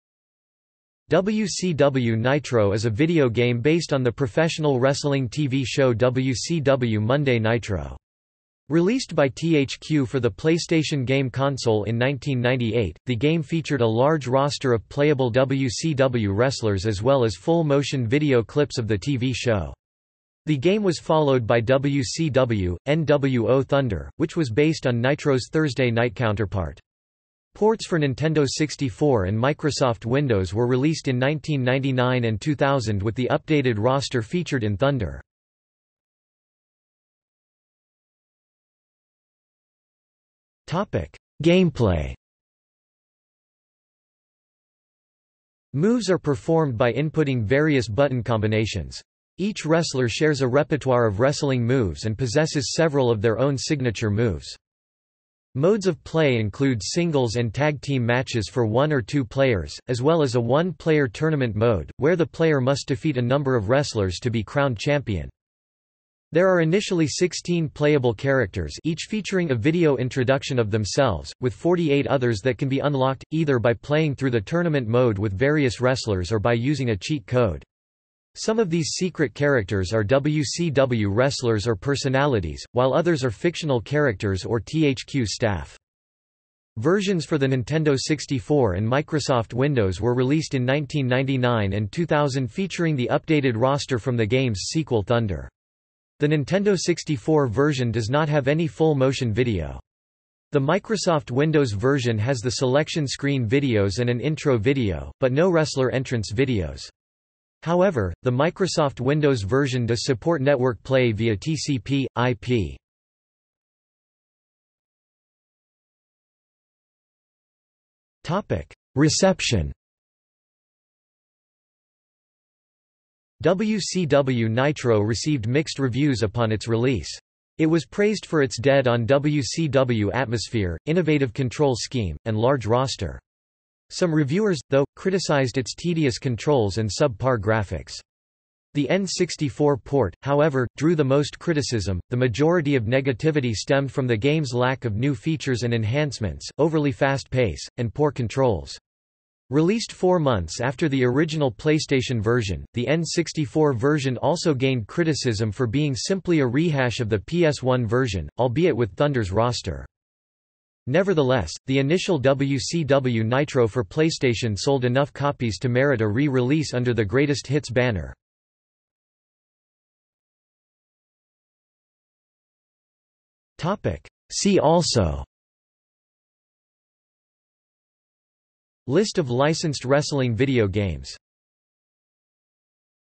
WCW Nitro (video game) | Wikipedia audio article
00:00:37 1 Gameplay
00:01:54 2 Reception
00:02:50 3 See also

- Socrates

SUMMARY
WCW Nitro is a video game based on the professional wrestling TV show WCW Monday Nitro. Released by THQ for the PlayStation game console in 1998, the game featured a large roster of playable WCW wrestlers as well as full motion video clips of the TV show. The game was followed by WCW/nWo Thunder, which was based on Nitro's Thursday night counterpart. Ports for Nintendo 64 and Microsoft Windows were released in 1999 and 2000 with the updated roster featured in Thunder.